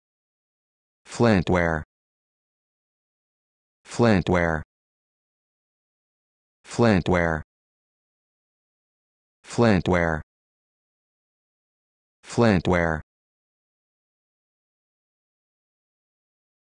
How to Pronounce Flintware
This video shows you how to pronounce Flintware